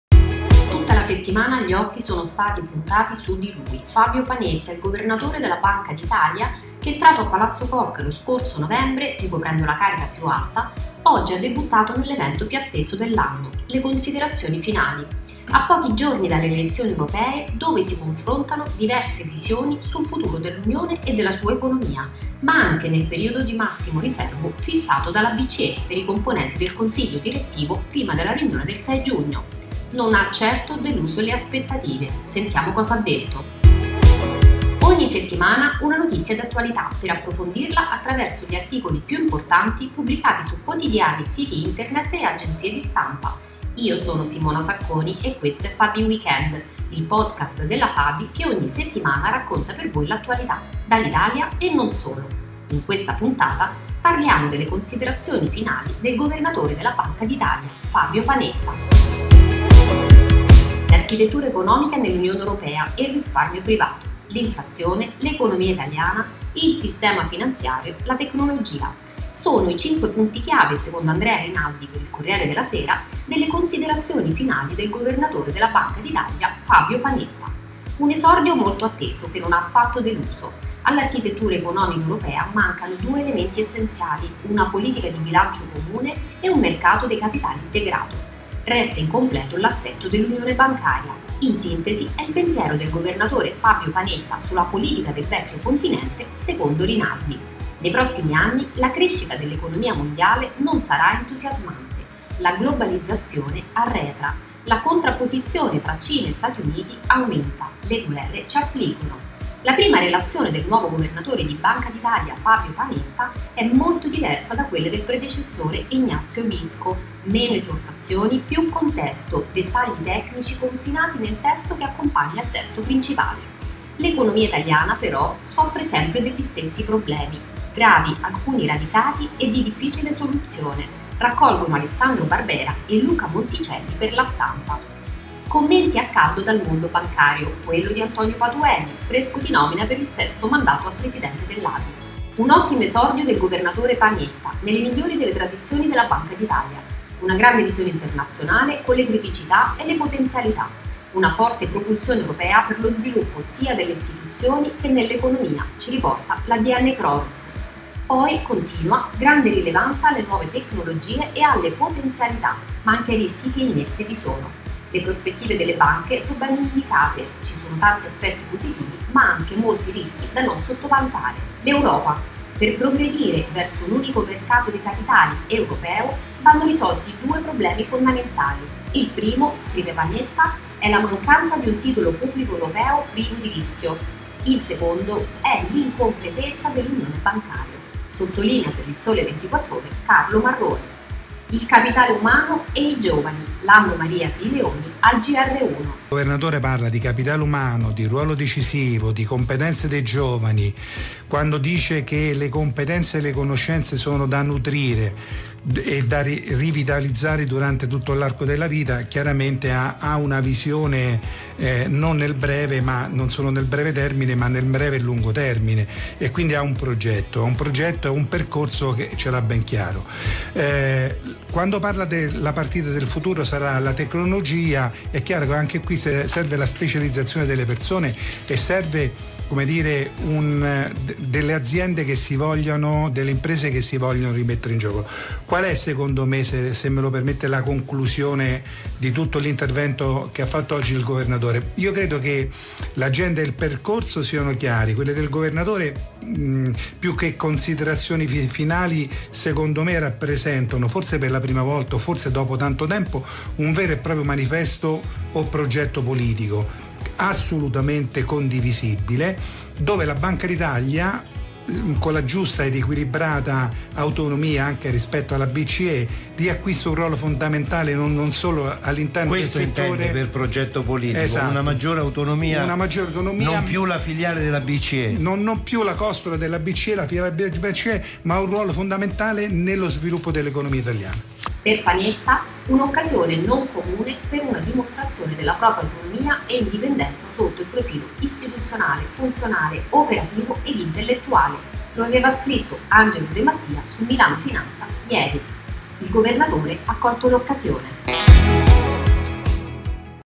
FABI WEEKEND - CONSIDERAZIONI FINALI DEL GOVERNATORE DELLA BANCA D'ITALIA
Tutta la settimana gli occhi sono stati puntati su di lui, Fabio Panetta, il governatore della Banca d’Italia che entrato a palazzo Koch lo scorso novembre da numero uno, oggi ha debuttato nell’evento più atteso dell’anno, le considerazioni finali. I passaggi più importanti raccolti Andrea Rinaldi, Alessandro Barbera e Luca Monticelli. E i commenti di Antonio Patuelli, raccolto da Adnkronos, e Lando Maria Sileoni in diretta su Gr1. A cura di Simona Sacconi.